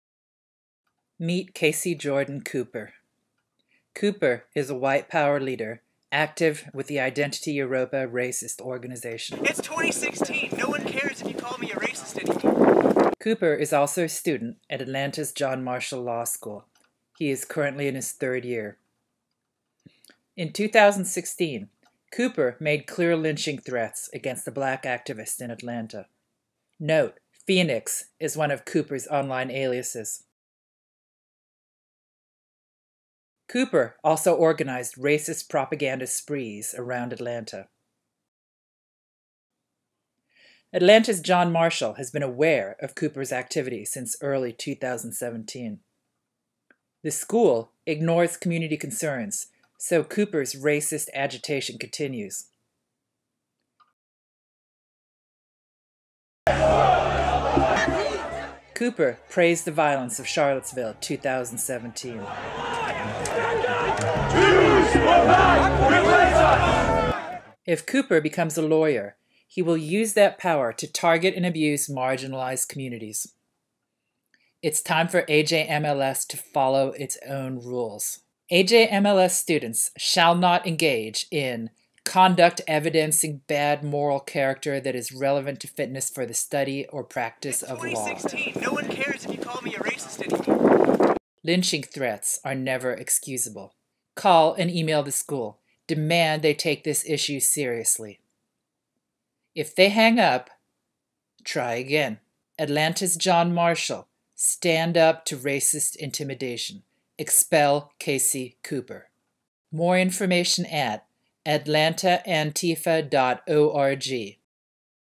Tell Atlanta's John Marshall Law School: Stand up to Racist Intimidation!
Casey Jordan Cooper is a student at Atlanta's John Marshall Law School responsible for white power organizing, unequivocal lynching threats, and racist propaganda campaigns. AJMLS has known about Cooper's activity for year and a half but ignores community concerns. Contact AJMLS and speak out.

Please share on social media and within your organization.